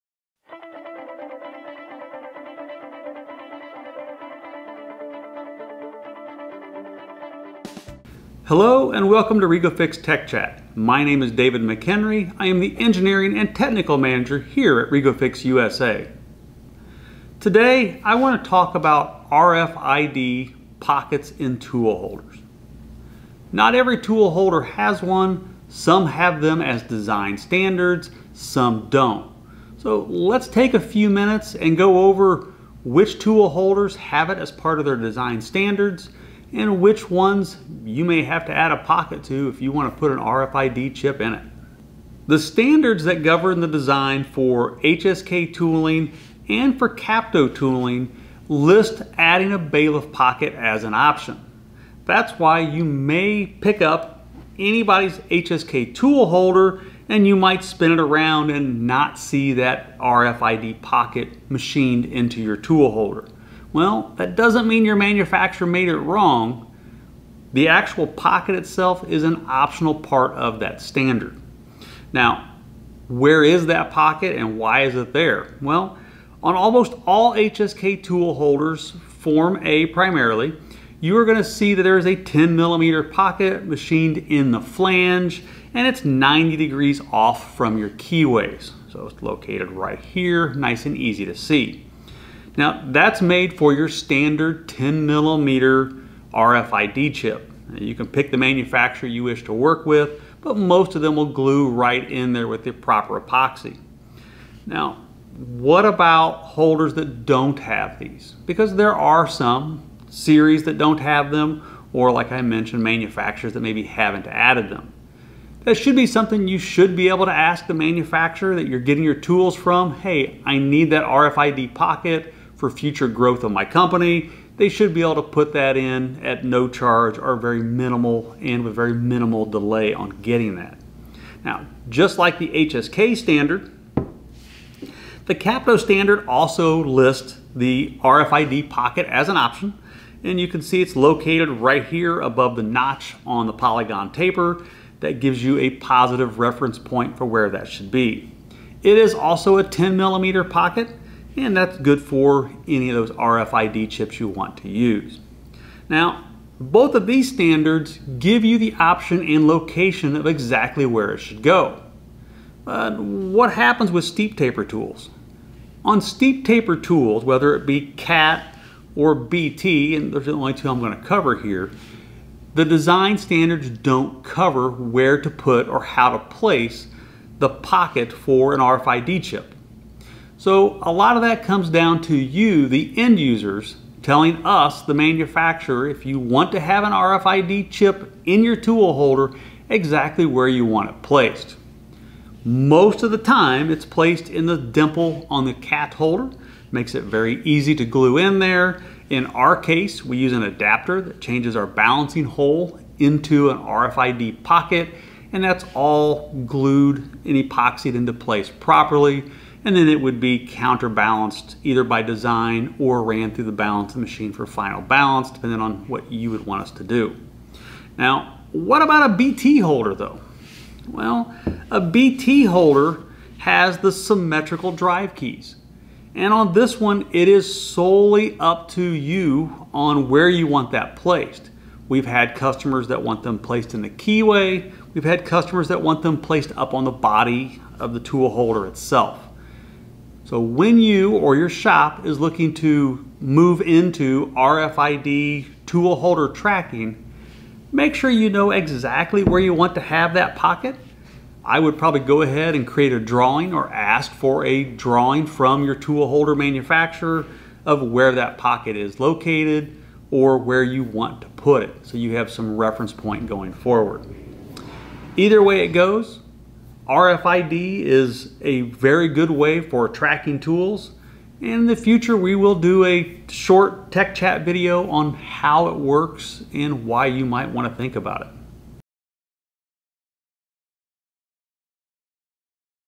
TechChat - RFID Chip Pockets
RFID is becoming more and more popular as automation and tracking becomes a necessity in many shops. David speaks a bit on RFID chip pockets, how they work with REGO-FIX toolholders, and what Industry 4.0 can do for you.